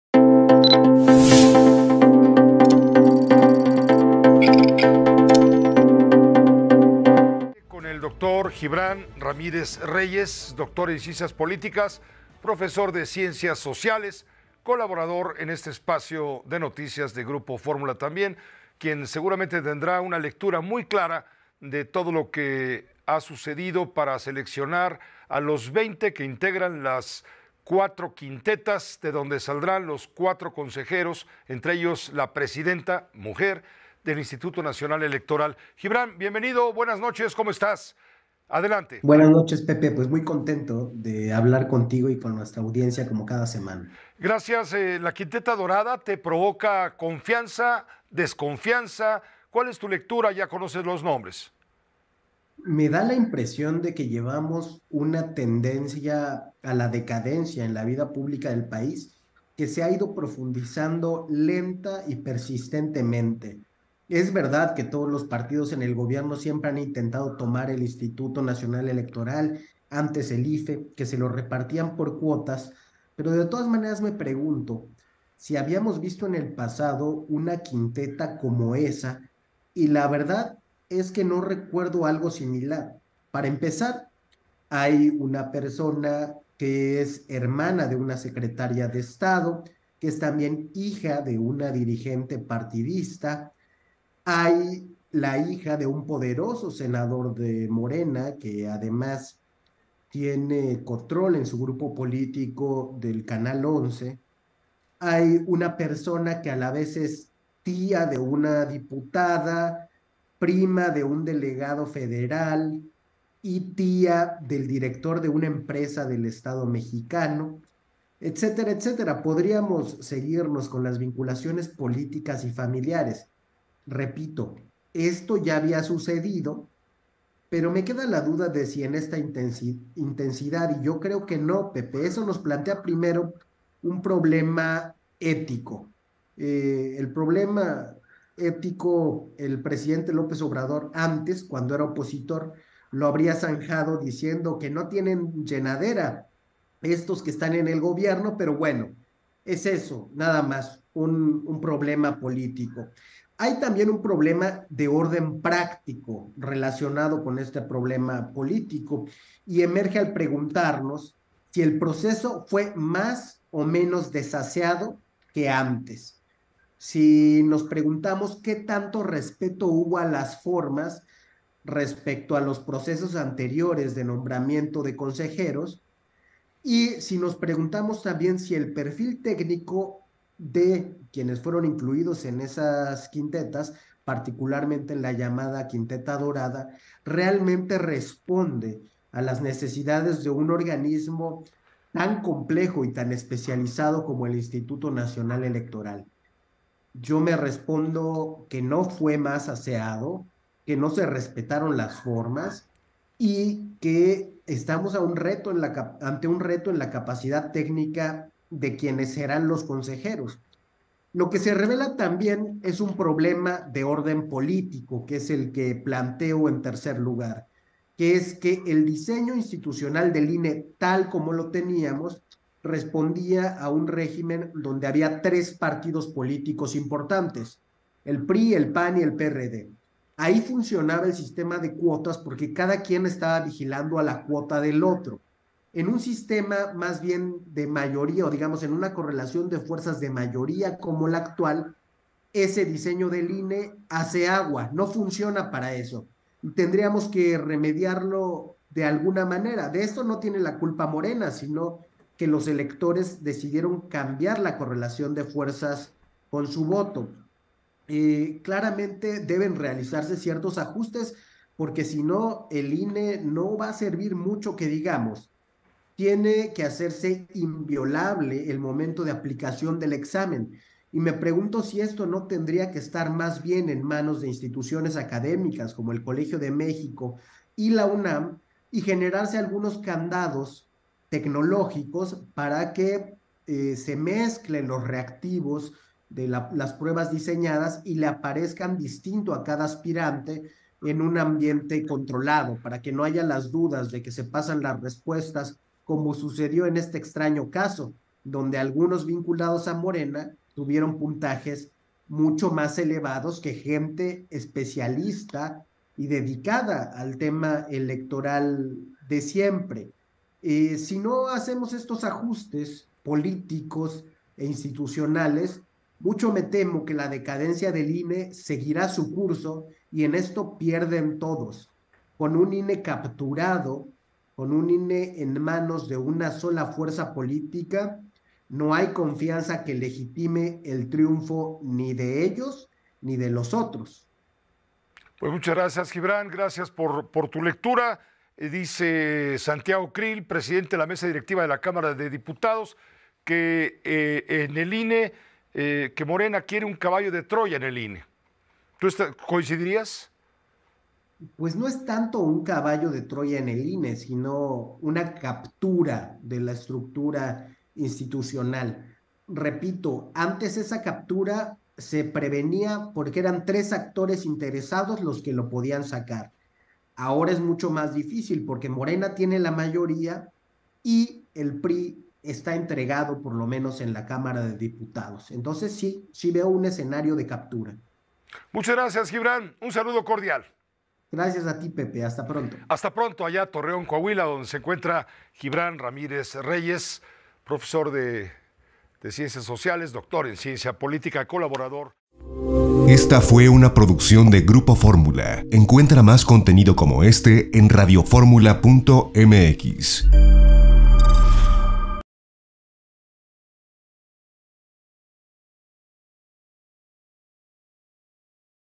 Consejeros del INE se definirán por tómbola: Gibrán Ramírez Reyes
Gibrán Ramírez Reyes, doctor en ciencias políticas y profesor de ciencias sociales, consideró que varios de los perfiles considerados para ser nuevos consejeros del INE están estrechamente ligados al Gobierno federal y a Morena. El partido político busca poner en marcha su Plan C, para tener consejeras y consejeros del INE a modo. En tanto, Morena y el PRI han calculado y preferido que la elección de cuatro nuevos consejeros y consejeras para el Instituto se realice por una tómbola. Cabe recordar que la Constitución explica que el procedimiento puede terminar en insaculación, el cual significa hacer una elección por tómbola, escenario que es probable en estos momentos debido al proceso y los plazos legales.